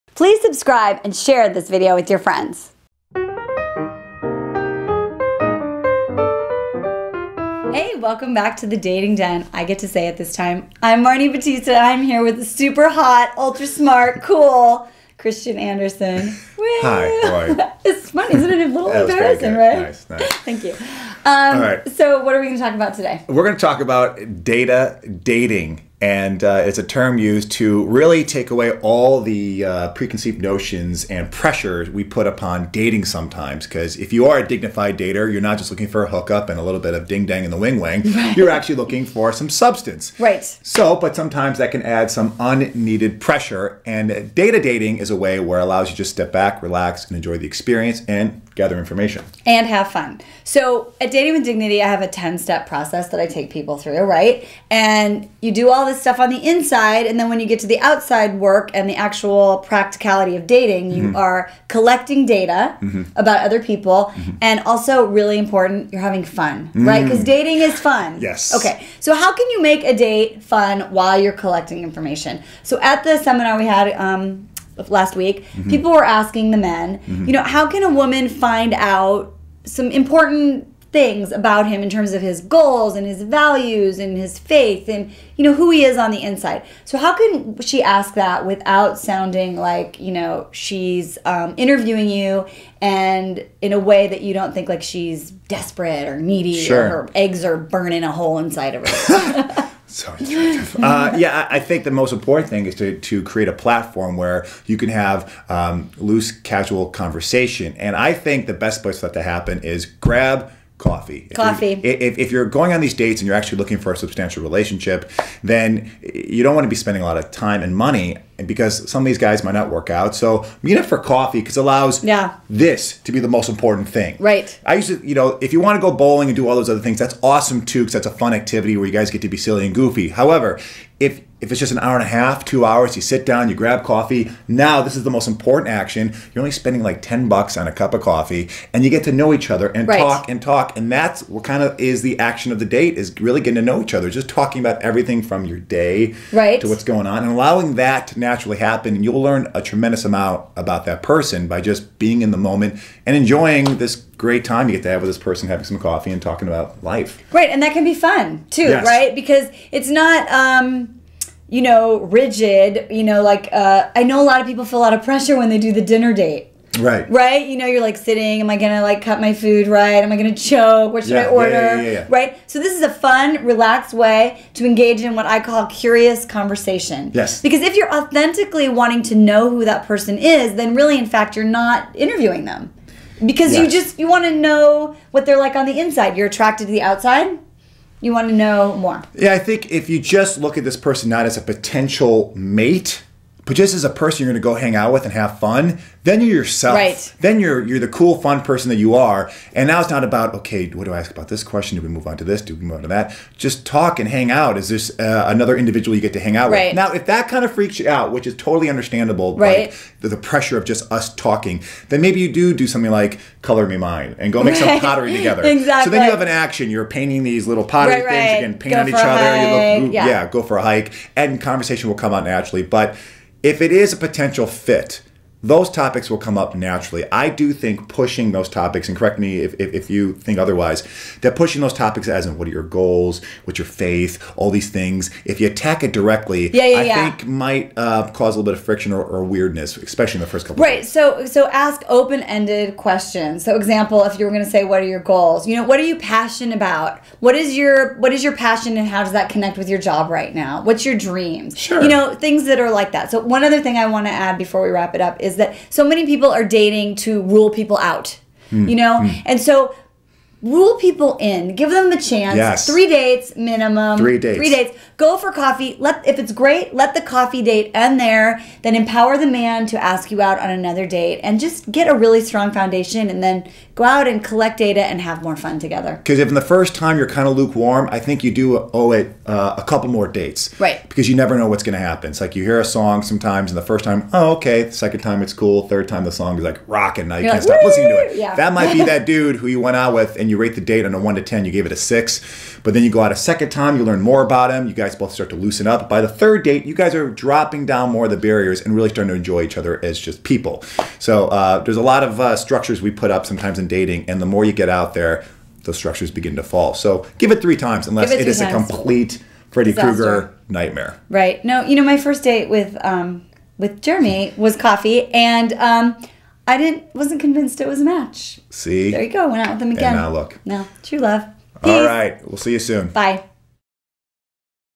How to Date Successfully: Become a Data Dater!
In this Dignity Dating Den episode, Dating With Dignity coach Marni Battista & Dignity Dating Man Panelist Christian Anderson give you dating advice, dating tips for women, tips for a first date and other tricks & techniques to become a successful Data Dater!

✅Follow Along On Marni and Jeremy’s Radical Living Challenge!